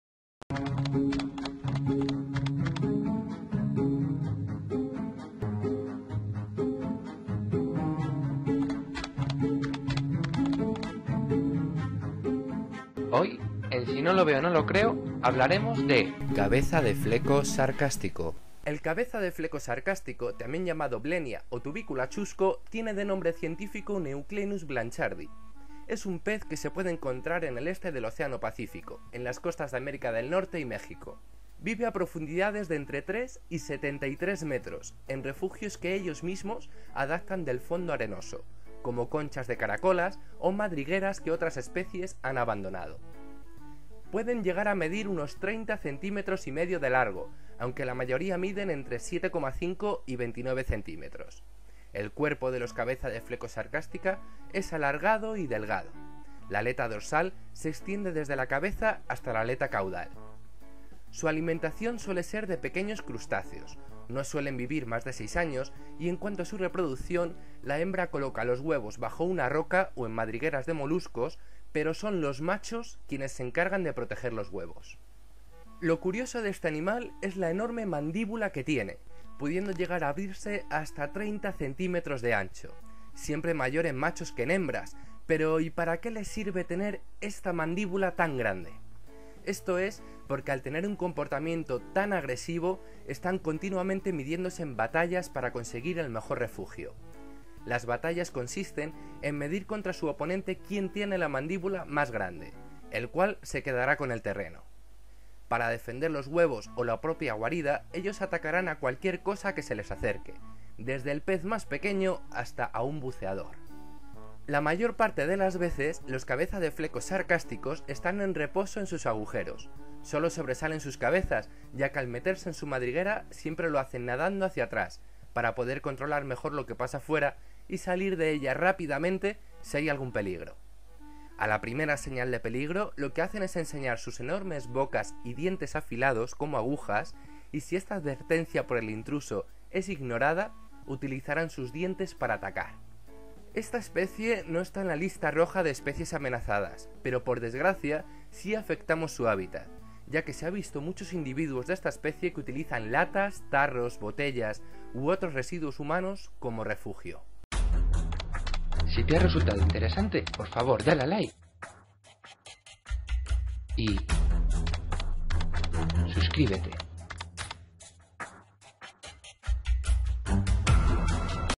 Cabeza de flecos sarcástico. La mandíbula extrema.
El cabeza de flecos sarcástico es un pez con una mandíbula extremadamente grande. ¿Quieres saber más de él? Mira el vídeo.